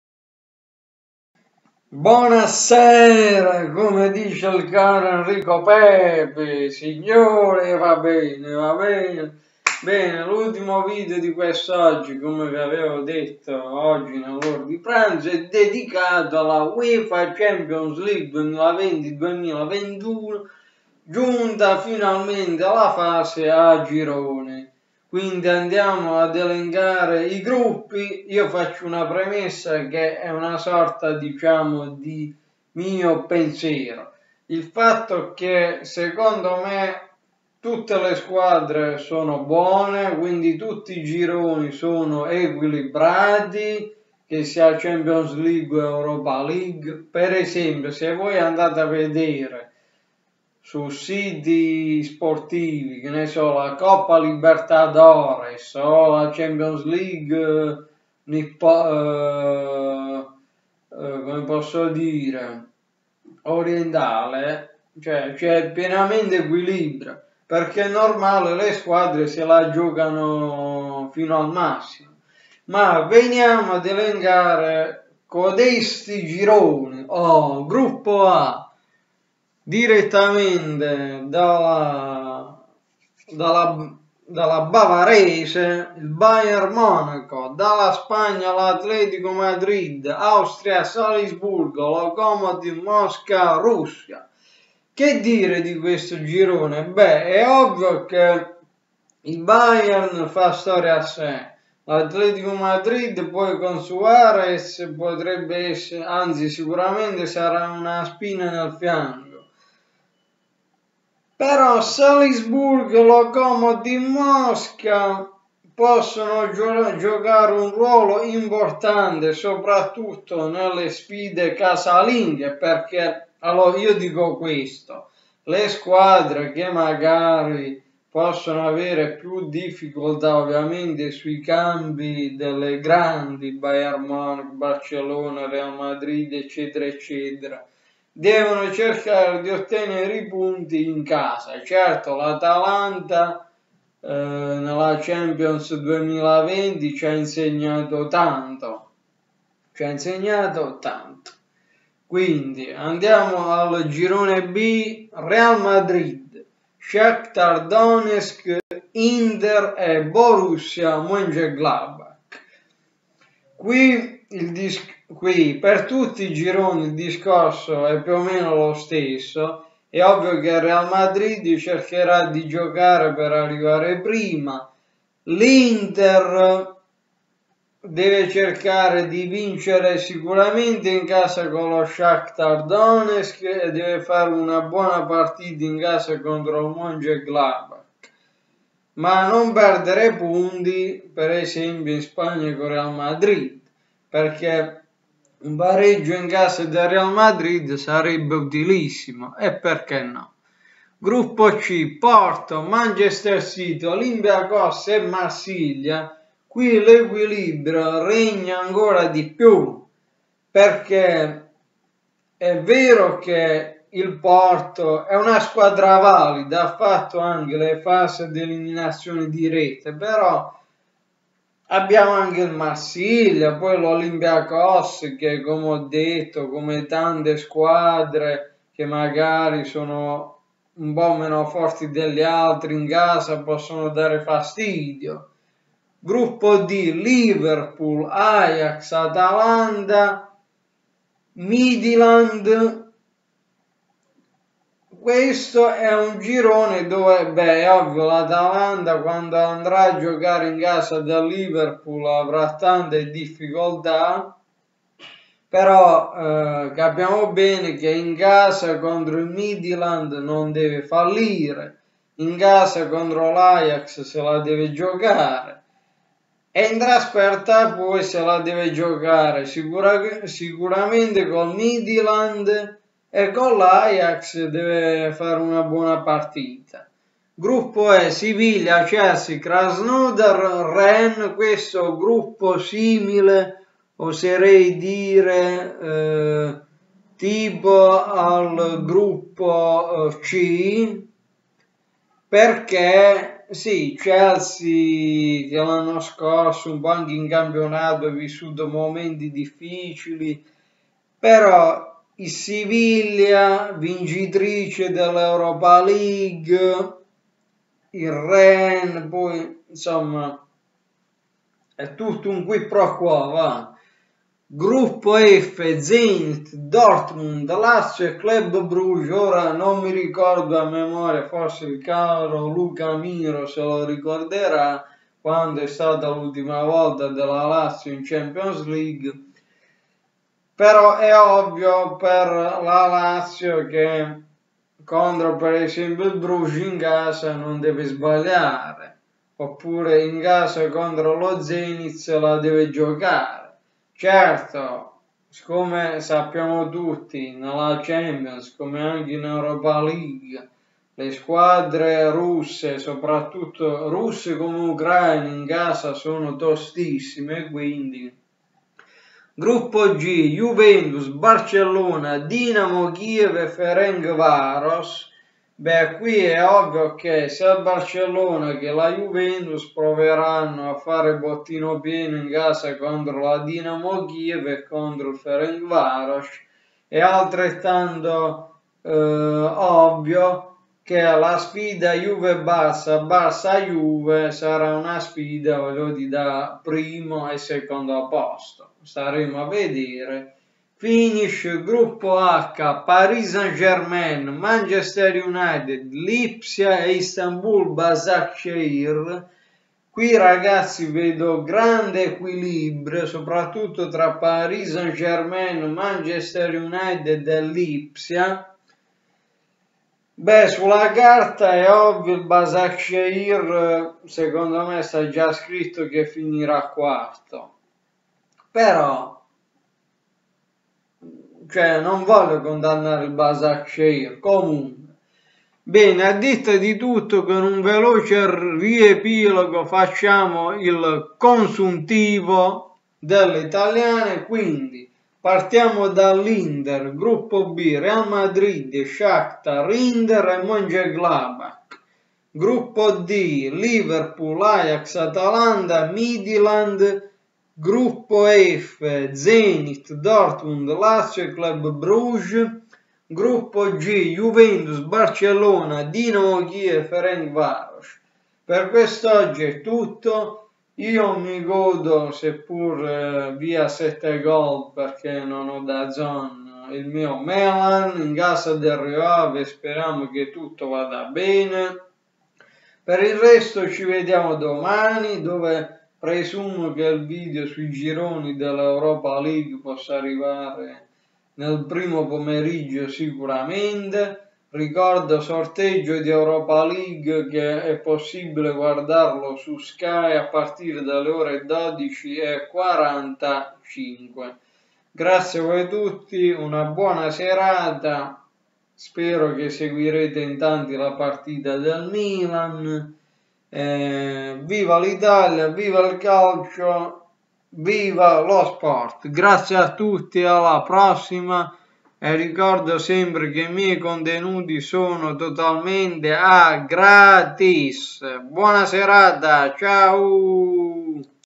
CALCIO UEFA CHAMPIONS LEAGUE 2021 FASE A GIRONI IL MIO COMMENTO POST SORTEGGIO
VIVA IL CALCIO E LA UEFA CHAMPIONS LEAGUE!!